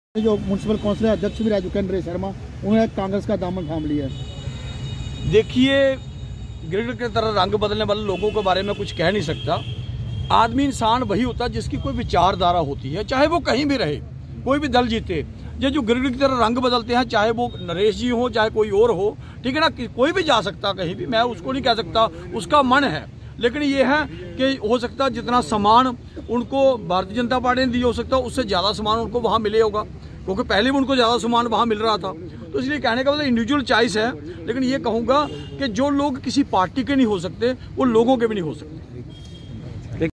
नरेश शर्मा द्वारा कांग्रेस पार्टी का दामन थामने के बाद आखिर यह क्या बोले भाजपा के वरिष्ठ नेता प्रेम
नरेश शर्मा द्वारा कांग्रेस पार्टी का दामन थामने के बाद आखिर यह क्या बोले भाजपा के वरिष्ठ नेता प्रेम नाथ डोगरा?